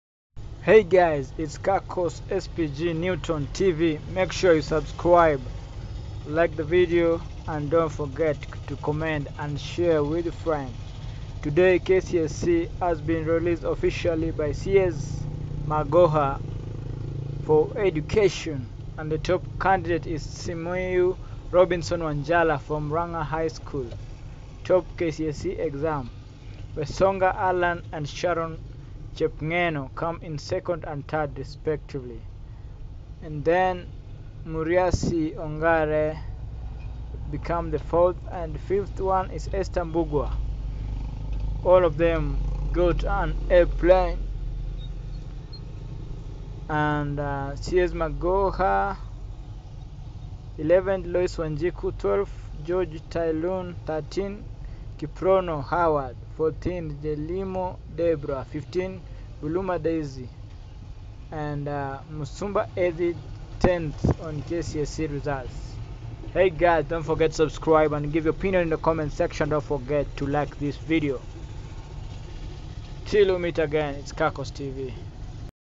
Top 10 KCSE candidate in 2021 exams Results Released Today by Cs Magoha
kcse result 2021
Top candidate in KCSE 2020 Countrywide!

Simiyu Robinson Wanjala from Murang'a High
kcse result 2021

CS Magoha: 893 candidates scored A plain. 223 candidates with special needs have obtained C+ & above. The most improved candidates are Cheruiyot Dennis, 190 in KCPE, B- now, Maluti Tom, 199 in KCPE, B- in KCSE, Kiptis Charity, 151 in KCPE, C+ in KCSE